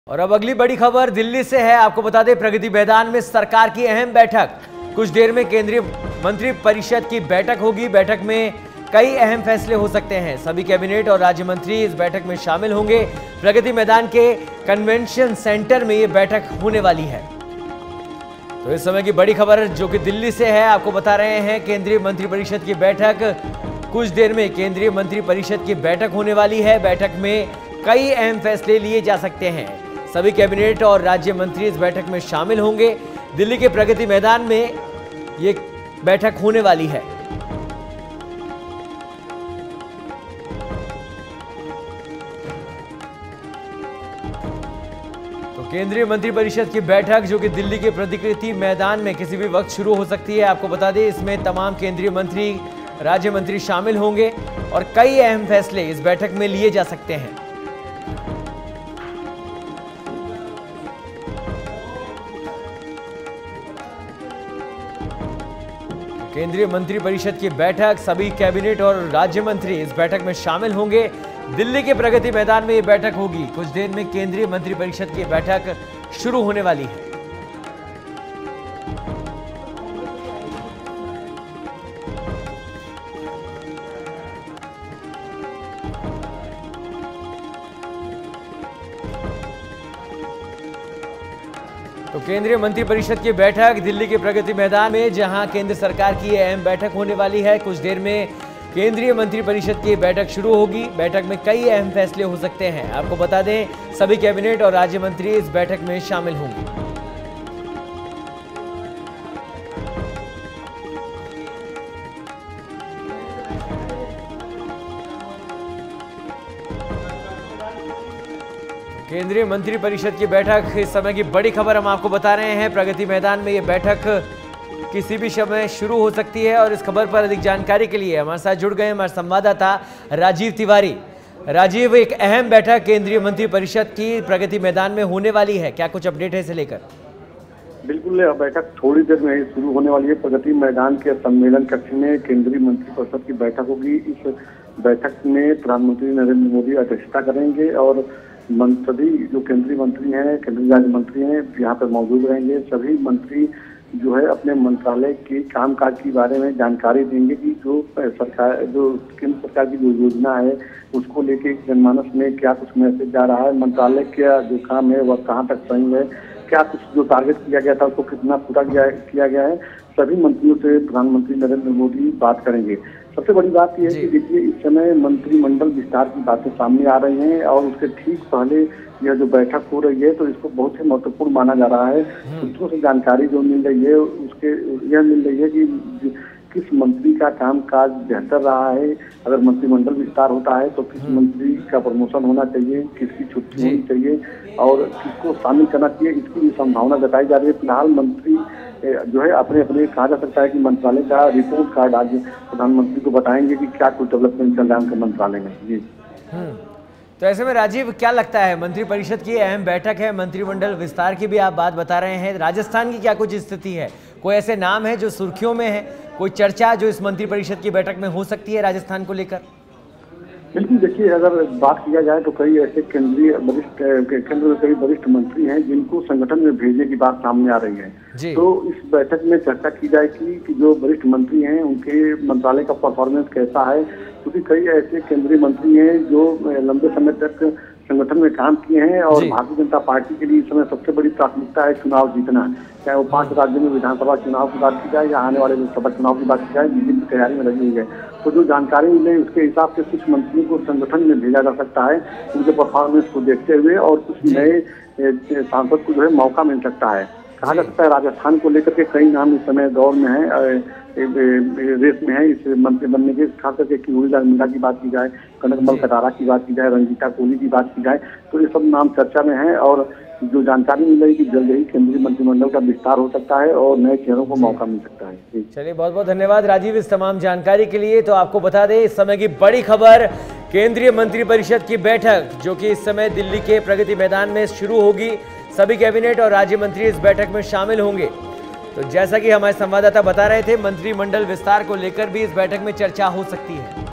दिल्ली के प्रगति मैदान में केंद्रीय मंत्रिपरिषद की अहम बैठक, हो सकते है अहम फैसले | Delhi News
About Video: दिल्ली के प्रगति मैदान में केंद्रीय मंत्रिपरिषद की अहम बैठक, हो सकते है अहम फैसले | Delhi News